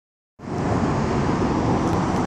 Audi S3 Turbo 20170220 181936
My sec S-Model Audi from 2/20/17. Has a Audi S3 w damaged on the driver's side.